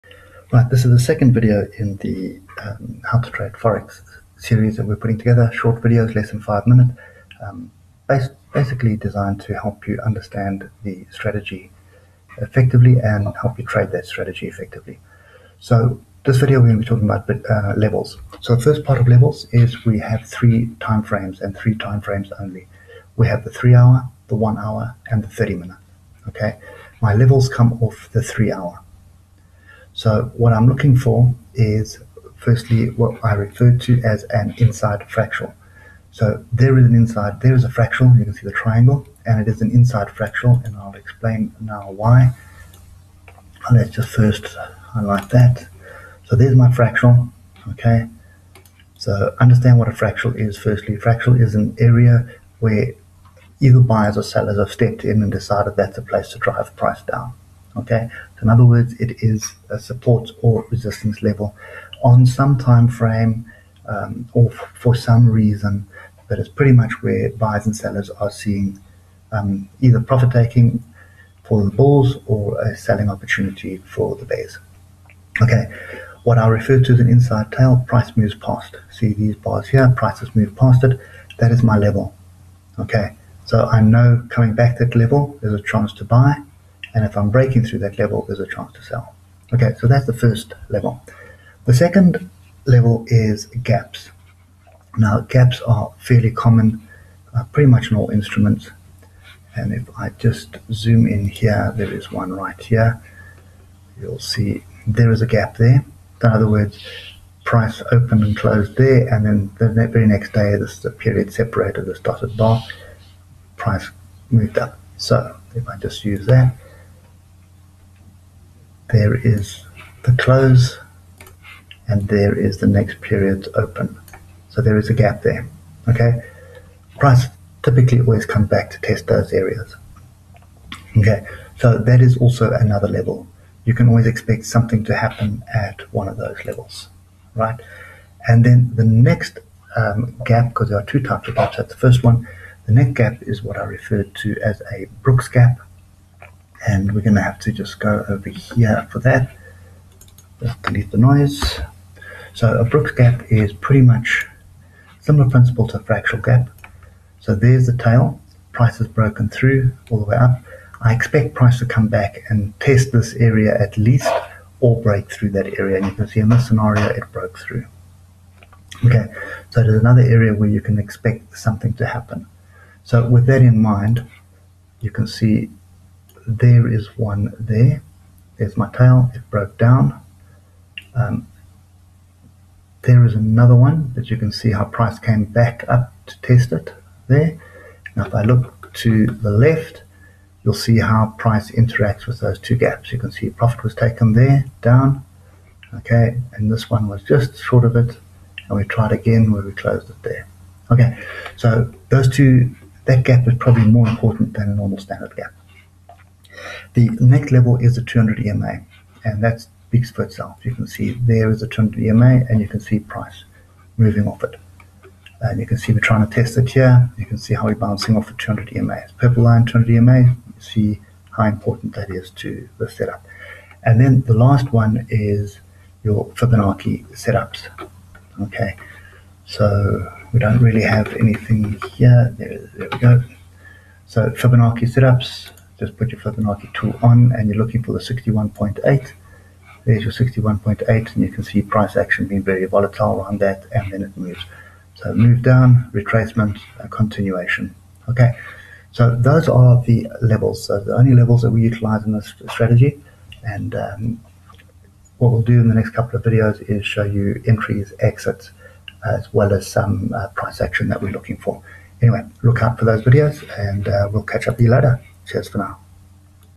How To trade Forex Dilligaf trend follow strategy, what are levels? Video 2
Video 2: How to trade Forex, following the trend series of videos.  In this video we look at levels.
3 Time frames:
30 minute
1 Hour
3 Hour

Levels:
Inside fractal
Gaps
200 exponential moving average
Fibonacci levels